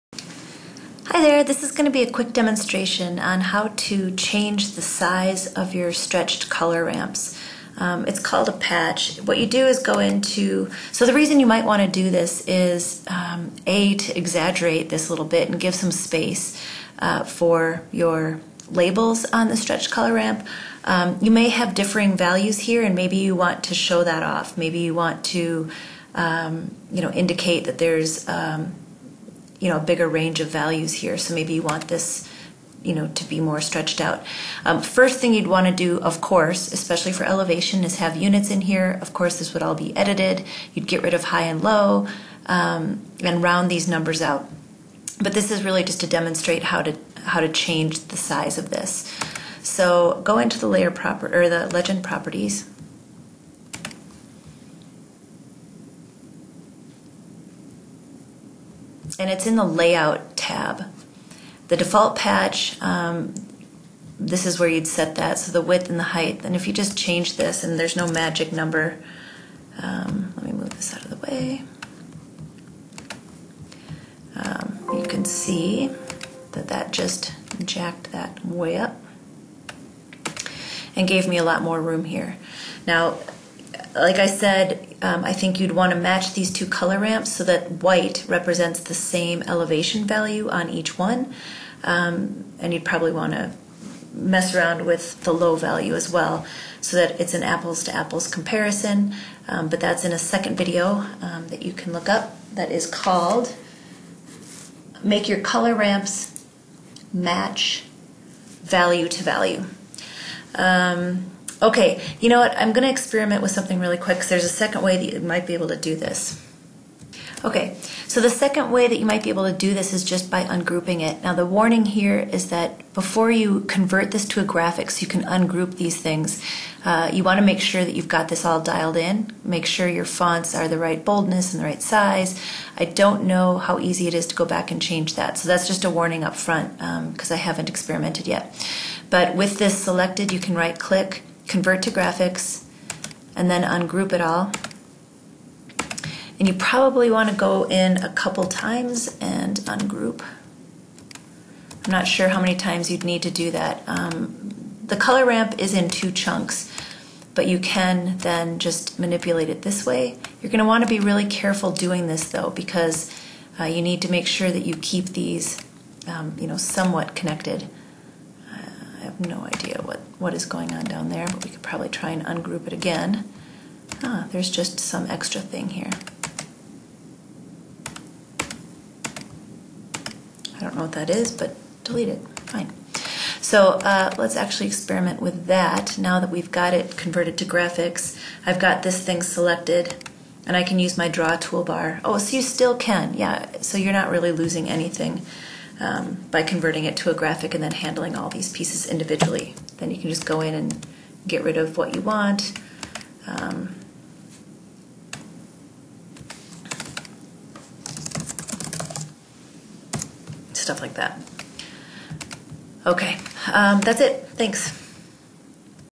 How to change the stretched ramp in your legend
Two methods, one changing the patch size in the legend properties, the other, converting to graphics and manually resizing.